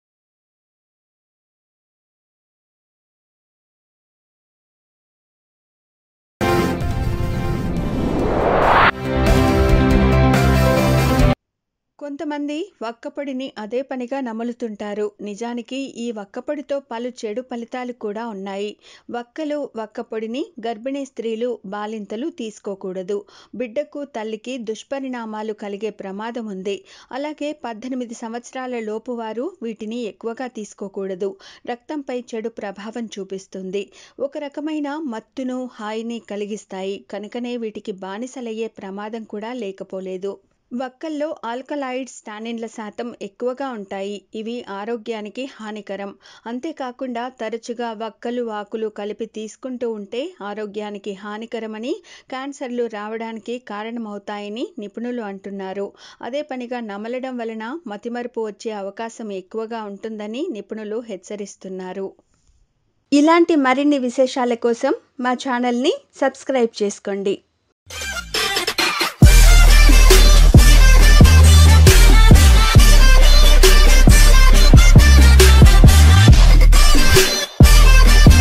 వక్కపొడి అదేపనిగా నమిలితే అంతే,అది చేసే చెడు ఏంటి. | Aarogya Sutra
Watch :వక్కపొడి అదేపనిగా నమిలితే అంతే,అది చేసే చెడు ఏంటి. | Aarogya Sutra
Aarogya Sutra